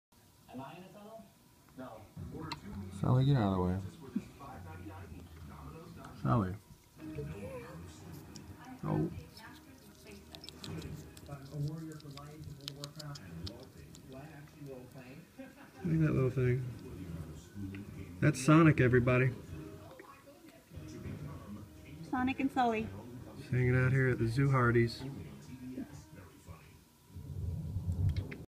Hedgehog
Our little Sonic. Hanging at the Zooharty's